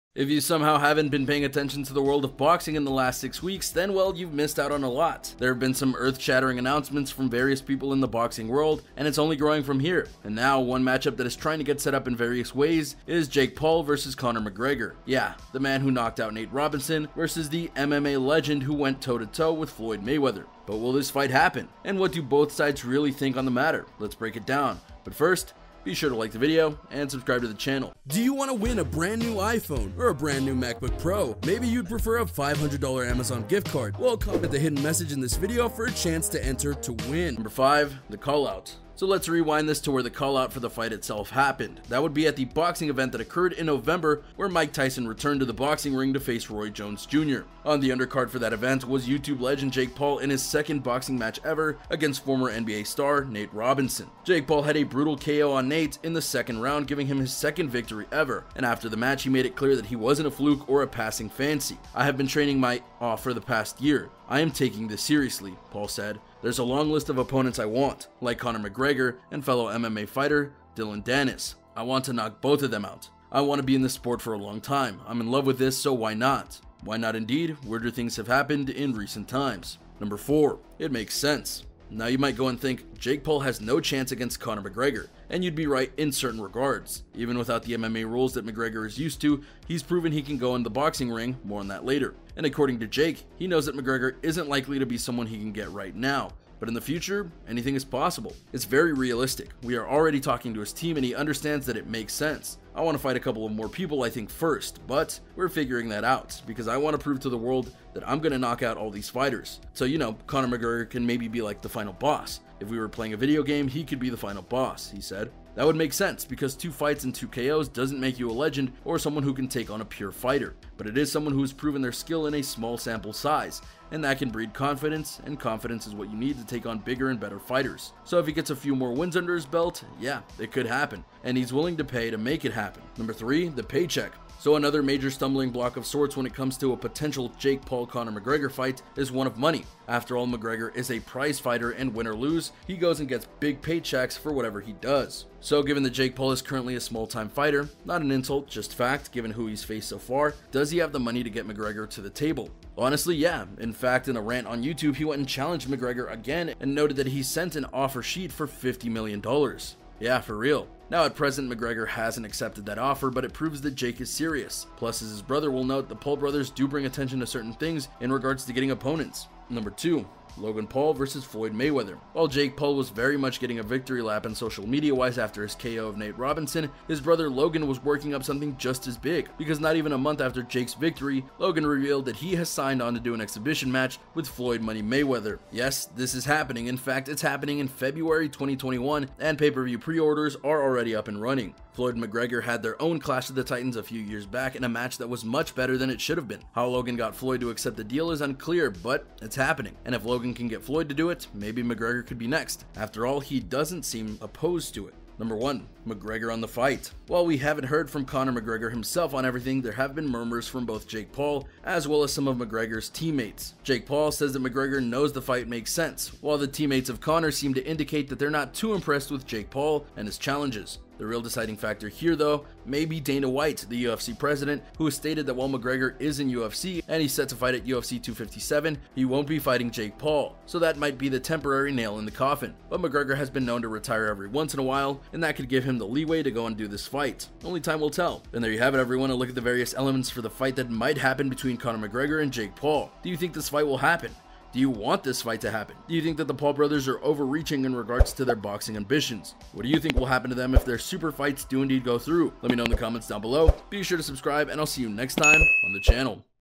FIGHT IS CONFIRMED Connor McGregor VS Jake Paul 2021...!?
If you somehow haven’t been paying attention to the world of boxing in the last six weeks then...well...you’ve missed out on a lot. There have been some earth-shattering announcements from various people in the boxing world and it’s only growing from here. And now, one matchup that is trying to get setup in various ways is Jake Paul vs Conor McGregor! Yeah, the man who knocked out Nate Robinson vs the MMA legend who went toe-to-toe with Floyd Mayweather. But will this fight happen? And what do both sides really think on the matter? Let’s break it down.

And there you have it, everyone! A look at the various elements for the fight that MIGHT happen between Conor McGregor and Jake Paul. Do you think this fight will happen? Do you WANT this fight to happen? Do you think that the Paul Brothers are overreaching in regards to their boxing ambitions? What do you think will happen to them if their superfights do indeed go through?